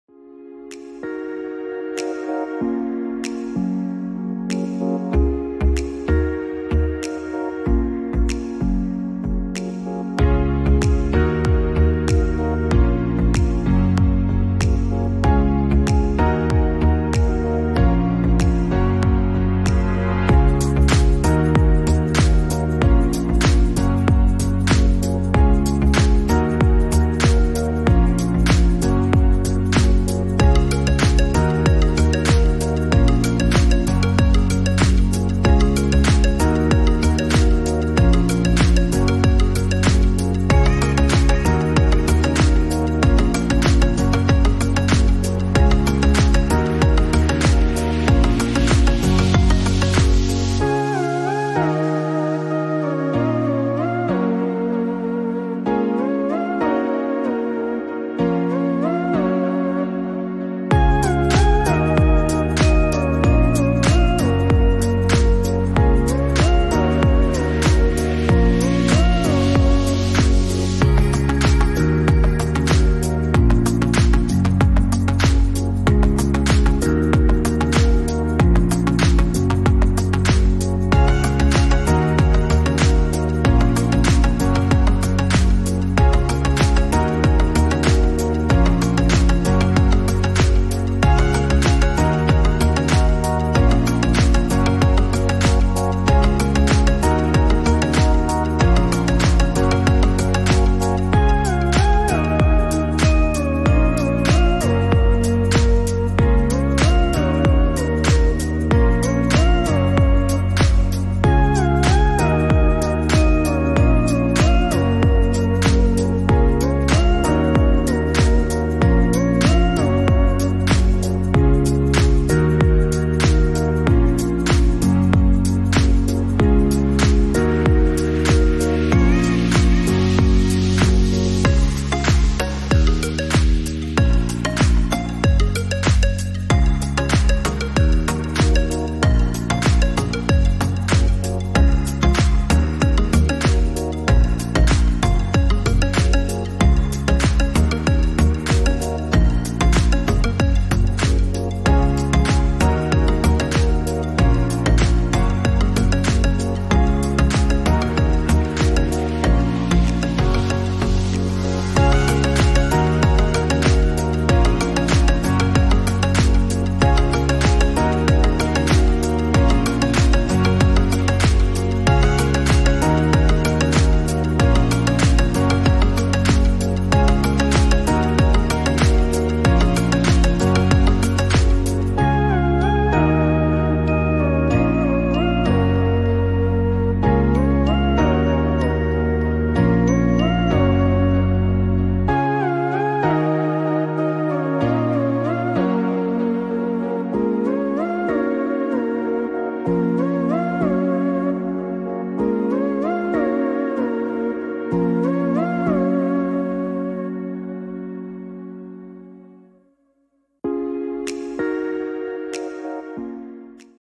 2022 Mini Cooper Electric Review, Pricing, and Specs
2022 Mini Cooper Electric Review, Pricing, and Specs

Connect 🔗 with MOAZZAMAUTO

This video is an overview of the 2022 Mini Cooper Electric  features, Price & specs.